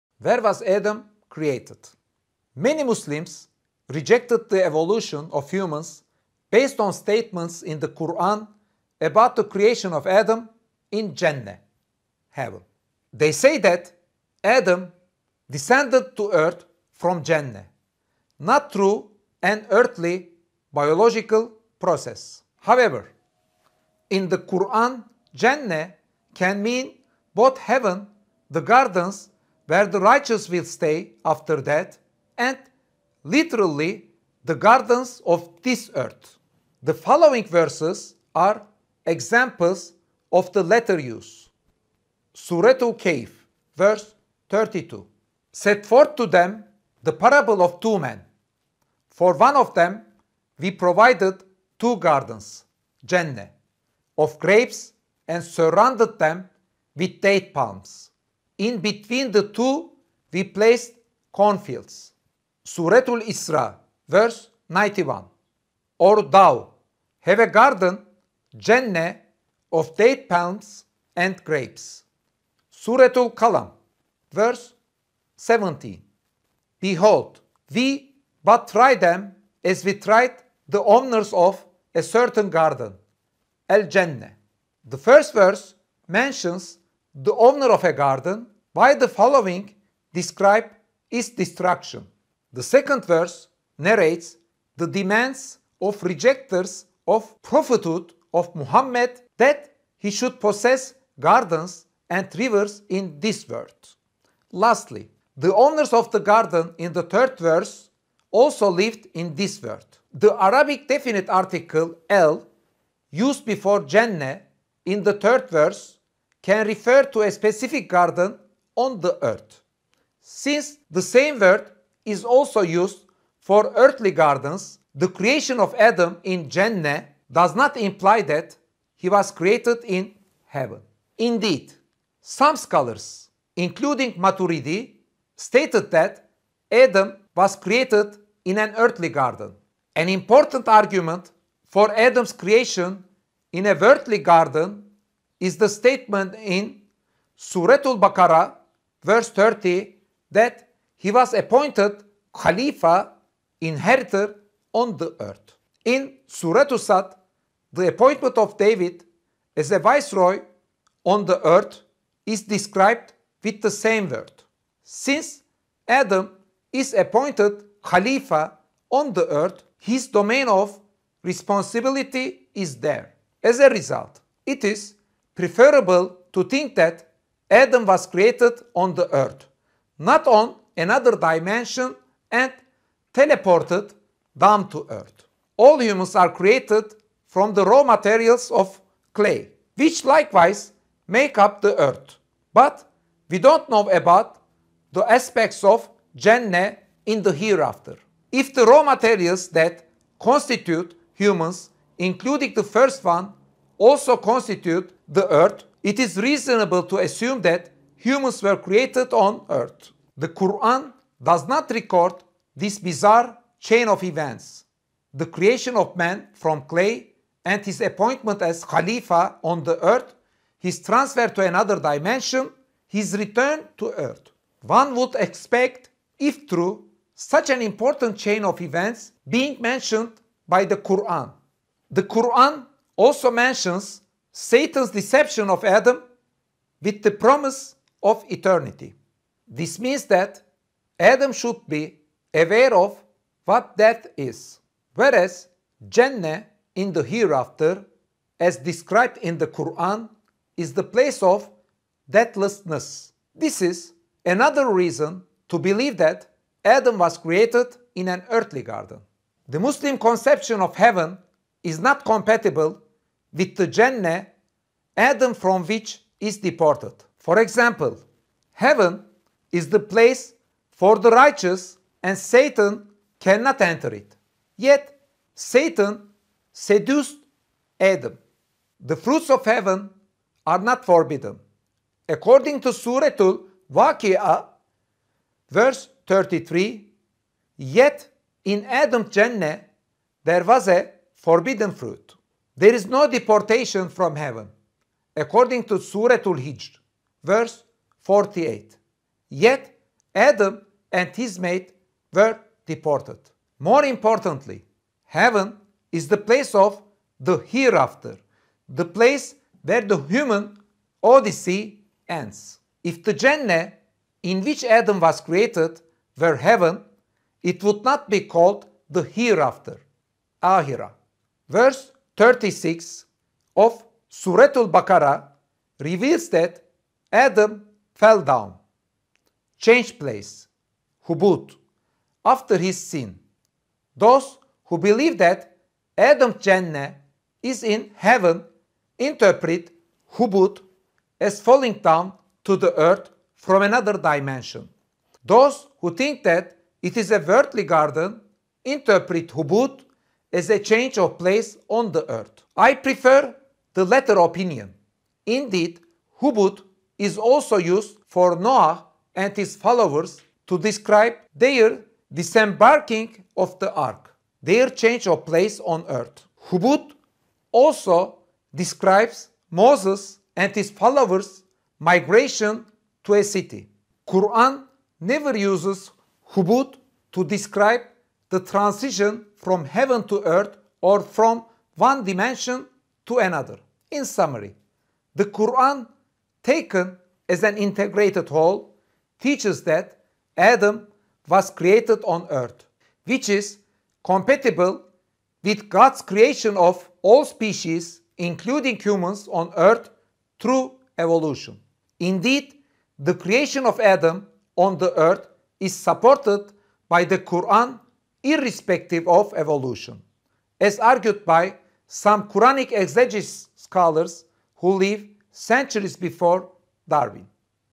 Where was Adam created | Prof. Caner Taslaman
Paradise means garden in Arabic. Prof. Caner Taslaman explains in detail with the verses of the Quran that the paradise where Adam was created is a garden in the world.